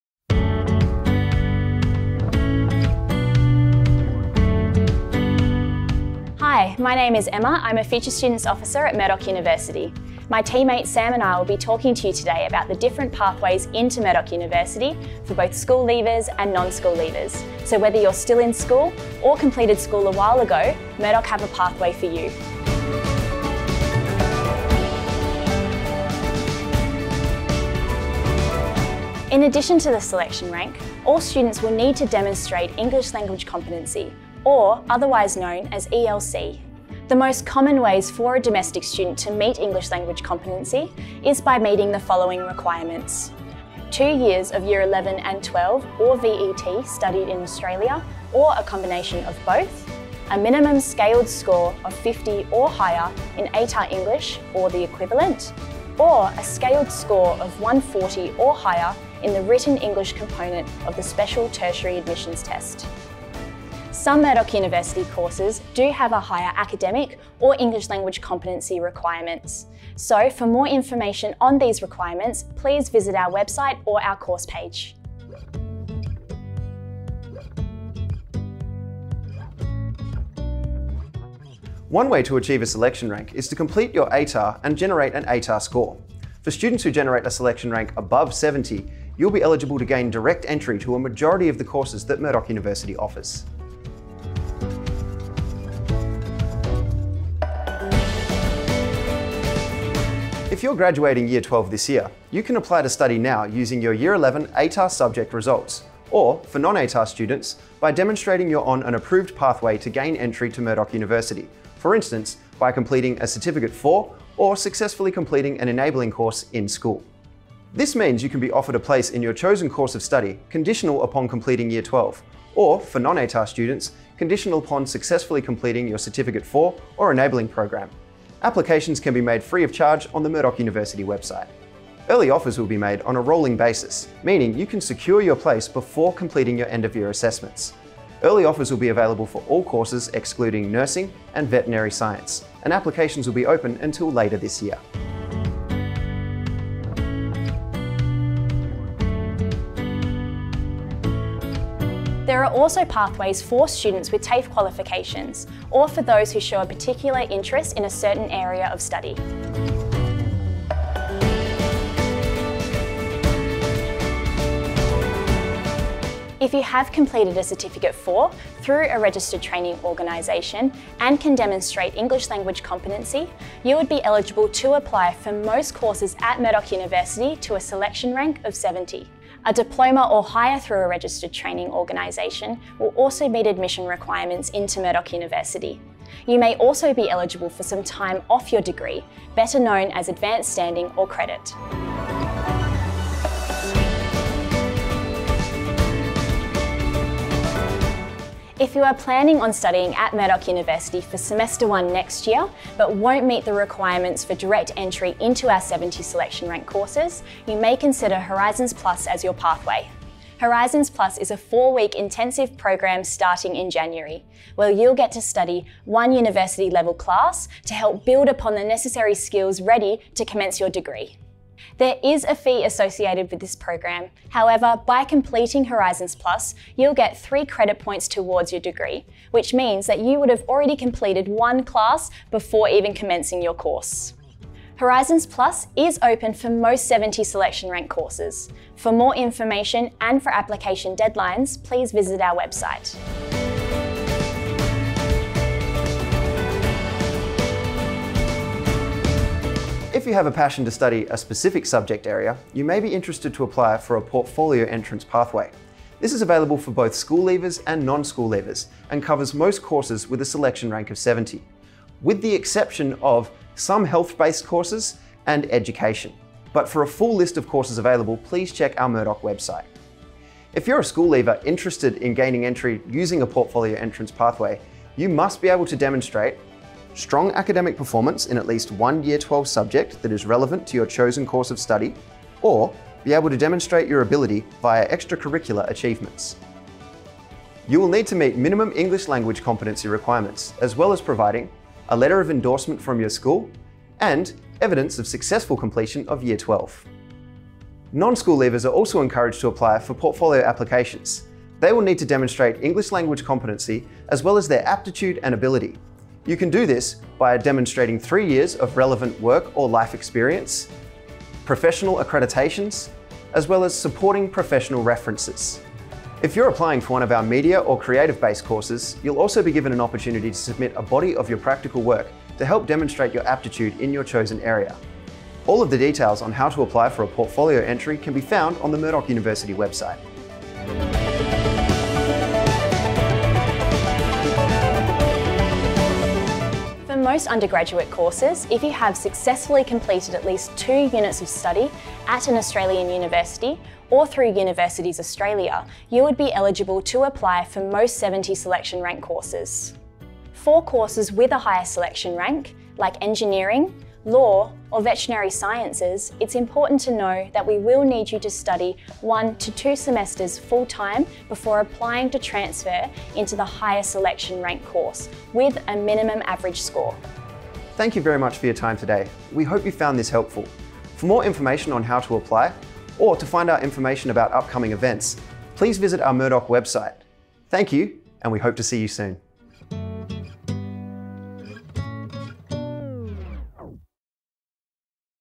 Admissions & Pathways into Murdoch University
Emma Pelliccione and Sam Macauley, Future Student Officers at Murdoch University, explain the range of admission pathways available for both school leavers and non-school leavers to gain entry into Murdoch University.